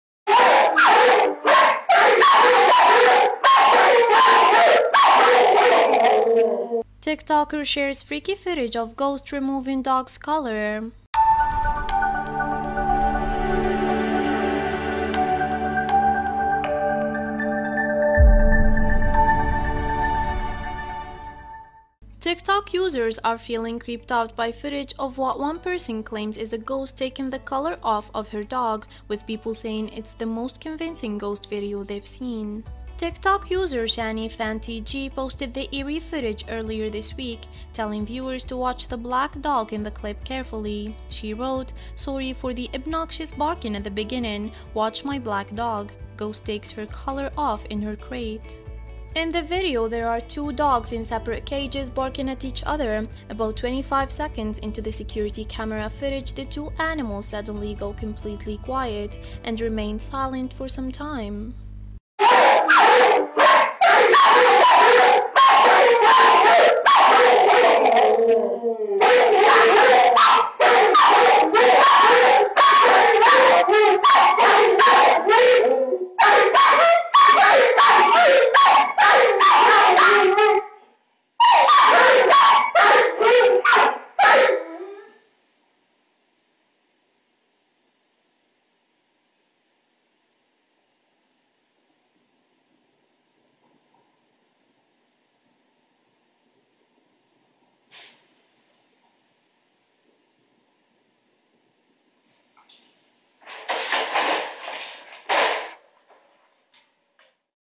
TikToker Shares Freaky Footage Of 'Ghost' Removing Dog's Collar!
TikTok users are feeling creeped out by footage of what one person claims is a 'ghost' taking the collar off of her dog - with people saying it's the most 'convincing' ghost video they've seen.
credit/ TikTok  : TikTok user Shannyfantg (@user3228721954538)